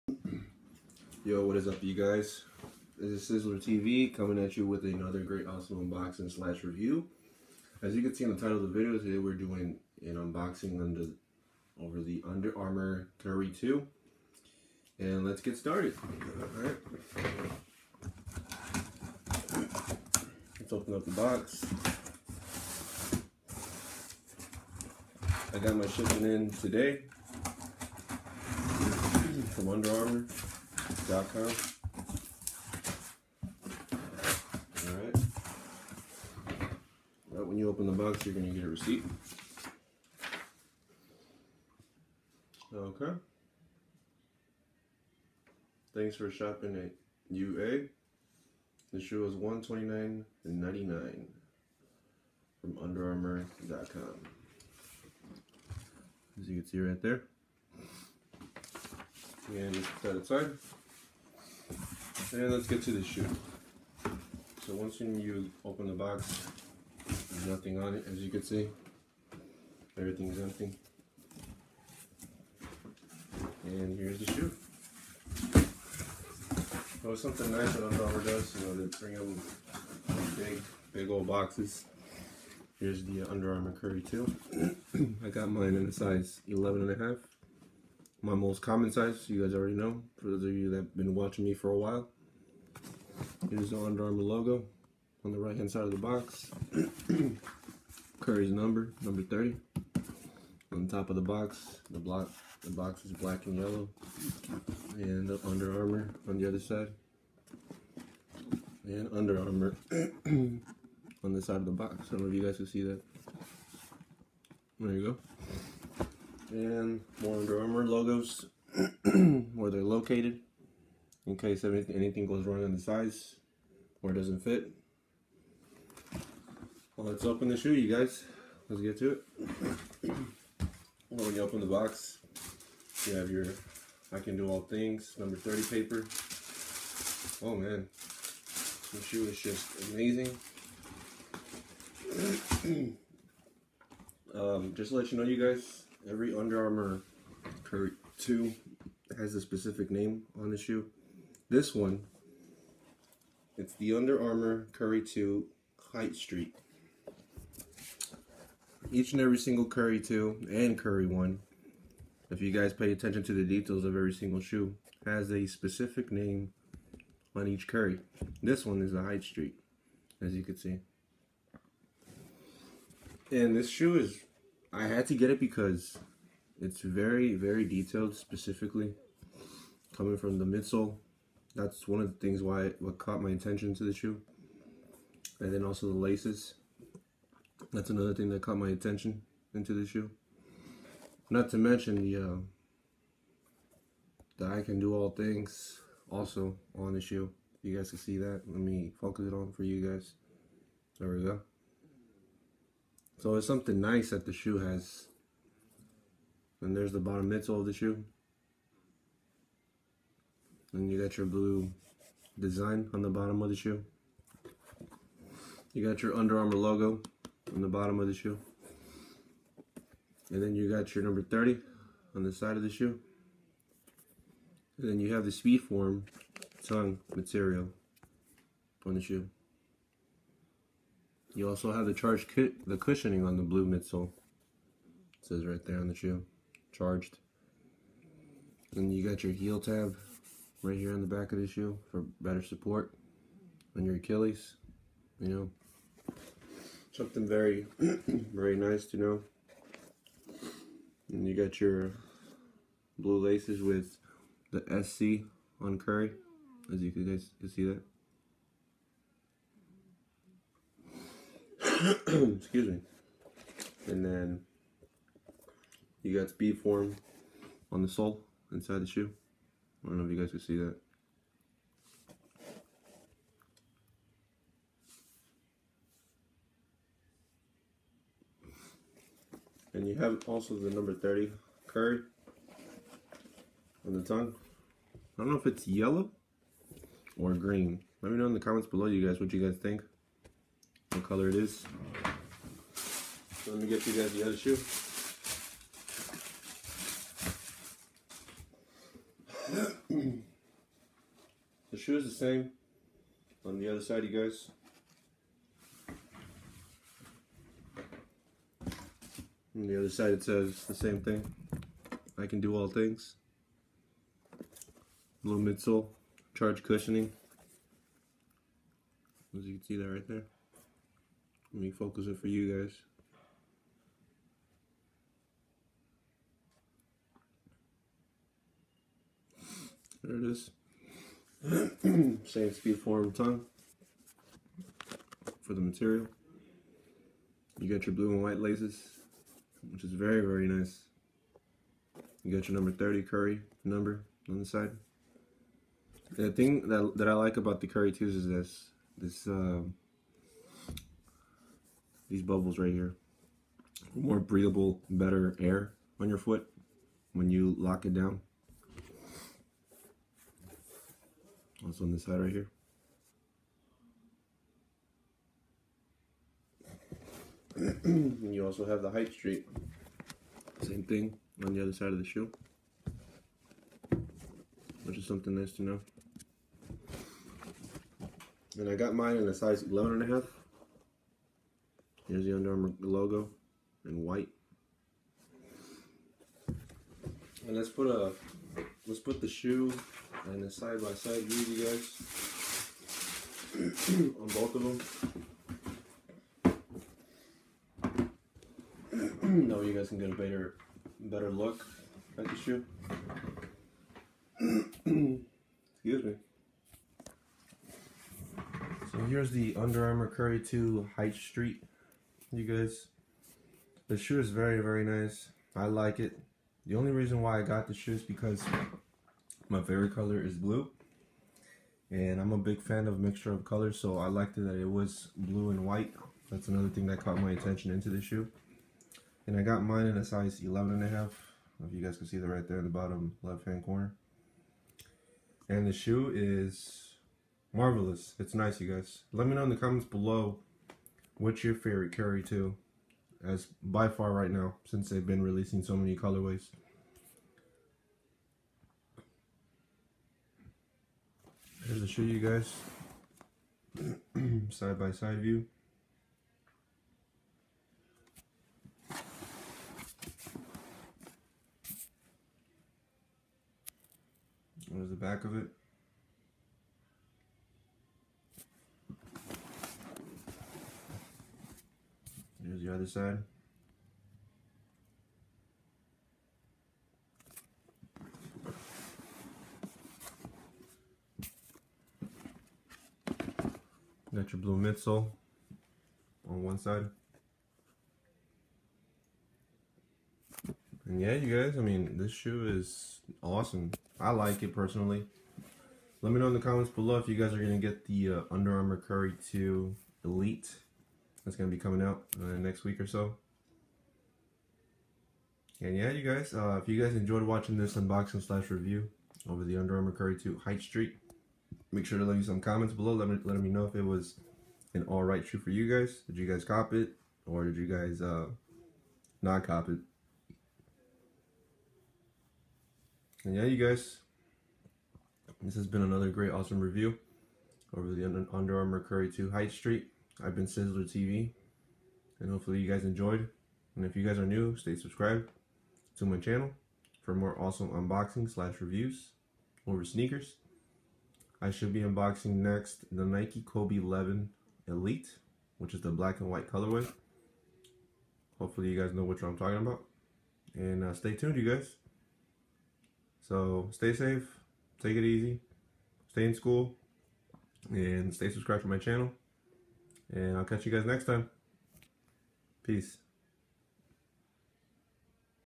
Under Armor Curry 2 Haight Street Unboxing/review
Bringing out another great unboxing/review for you guys, this time its the under armor curry 2 haight street.
Drop a like for more Curry 2's unboxing's!